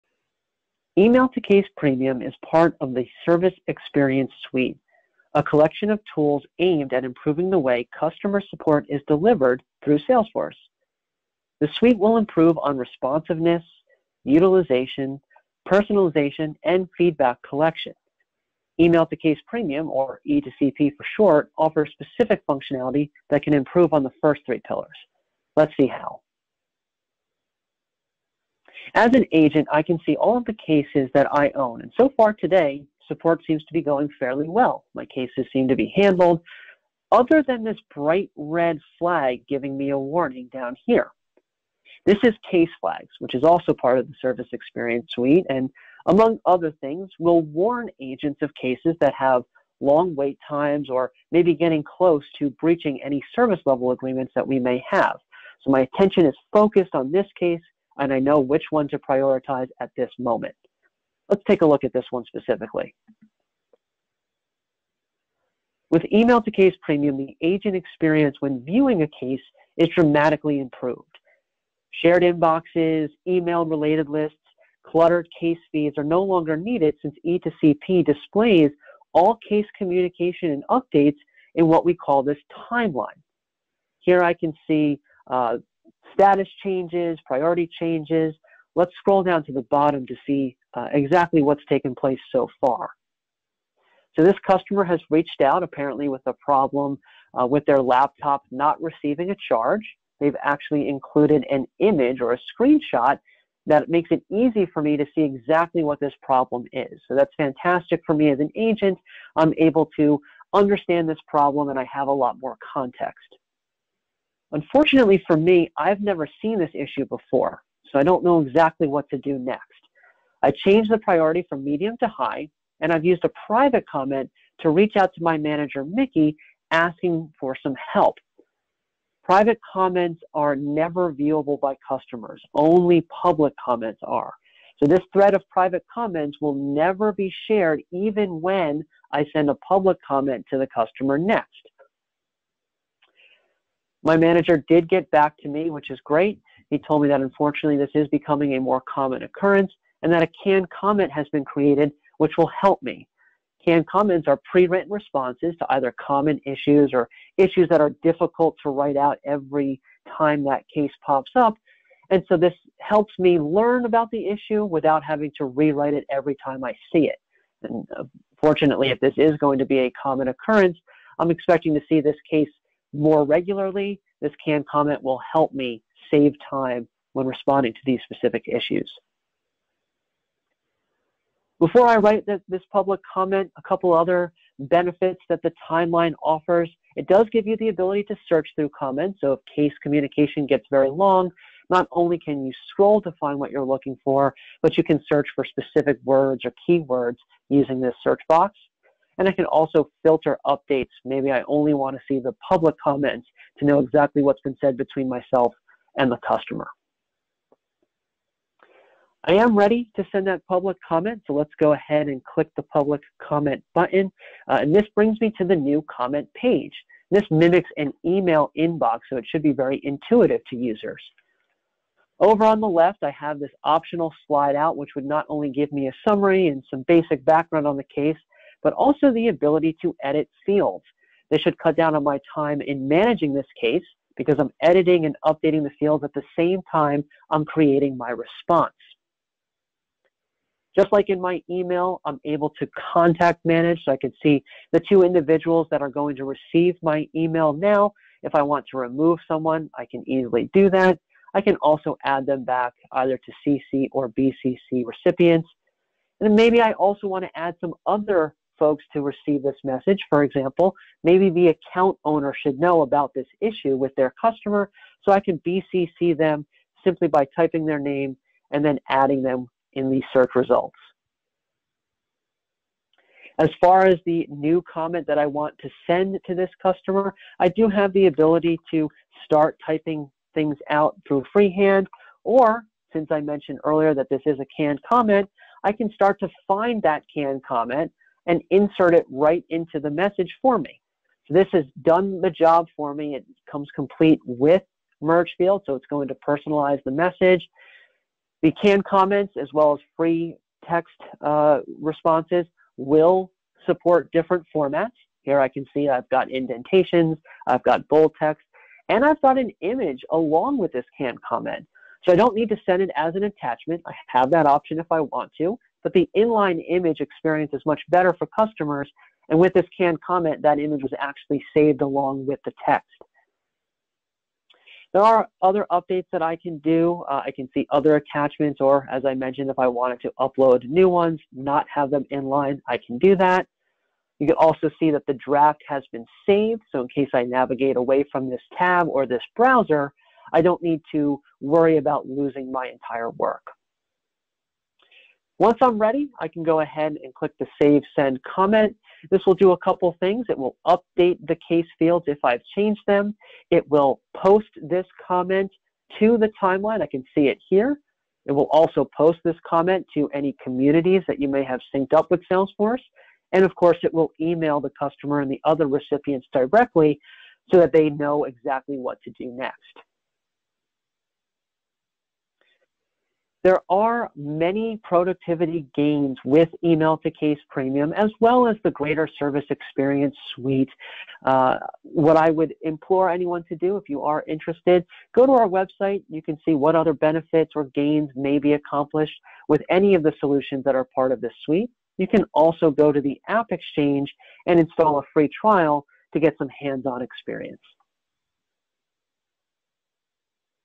Email to Case Premium for Salesforce - Service Agent Walkthrough
Email to Case Premium is a robust solution for managing cases in Salesforce and creates a better experience for service agents and customers.

This video shows the following:

0:00:00 Intro to Service Experience Suite and Email to Case Premium (E2CP)
0:00:38 Case Flags indicates a case that needs a response
0:01:05 How E2CP improves agent experience of viewing a case with Timeline
0:01:40 Hovering over an image attachment link shows a preview
0:01:59 Private comments for internal communication
0:03:03 Other benefits of Timeline, like filtering to find specific kinds of case updates
0:03:31 Functionality of New Comment Page for outbound communication
0:04:43 Canned Comments save time and create consistency
0:05:46 Save case updates & Send outbound communication in one step
0:06:13 For agent productivity gains, install a free trial on the AppExchange